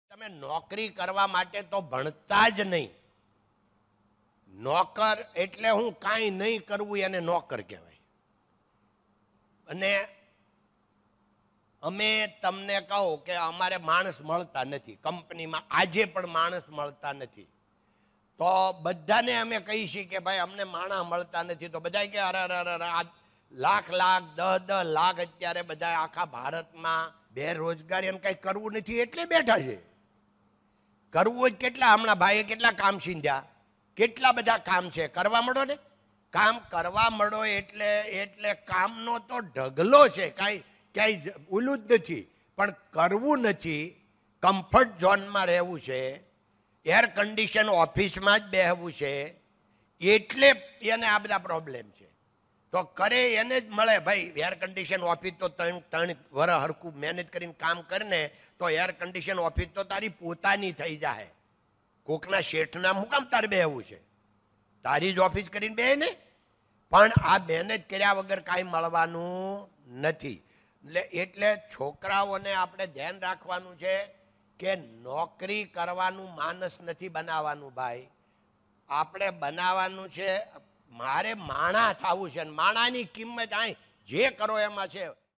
Govind Dholakia talks about stepping out of comfort zone for success
In this video Shri Govindkaka talks about how success comes from hard work and taking action instead of staying in our comfort zones. He also says that there are plenty of work one must try and gain practical example.